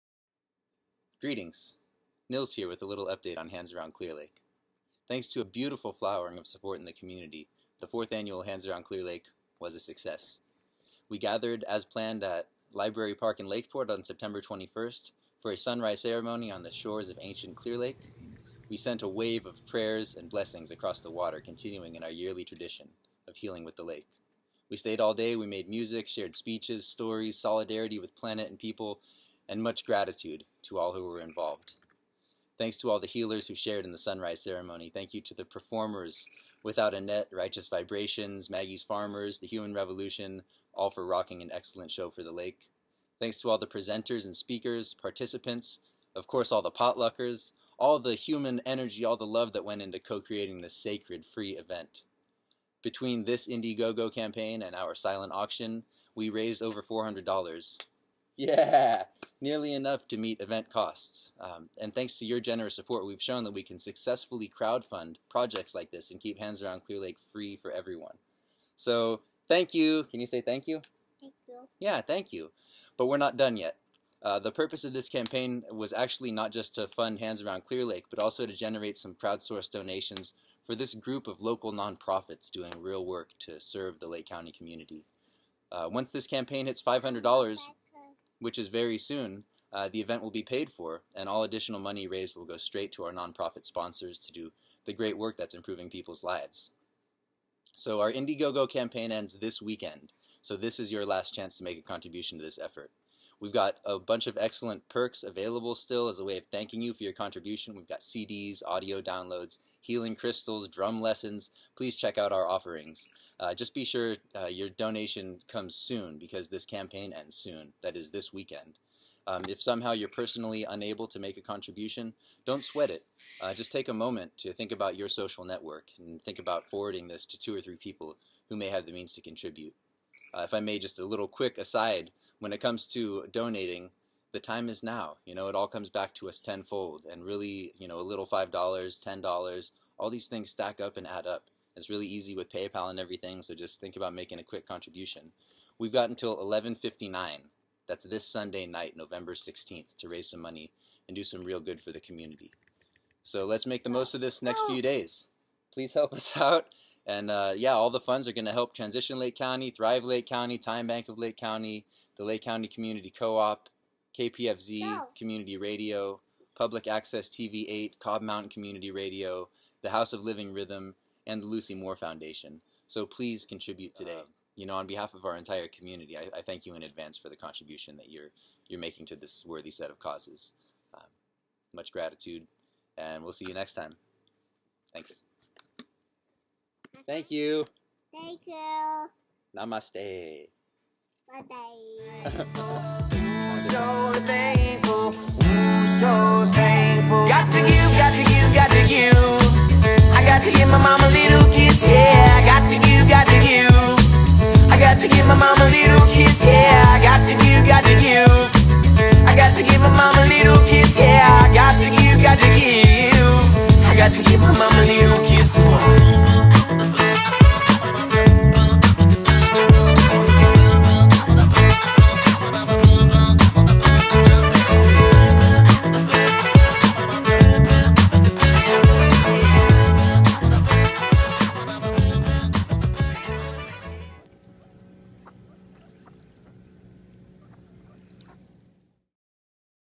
Hands Around Clear Lake 2014 Fundraiser Update
Hands Around Clear Lake's 2014 Indiegogo crowd-funding campaign ends this weekend -- Sunday, November 16, 2014 at 11:59PM! Please consider making a contribution.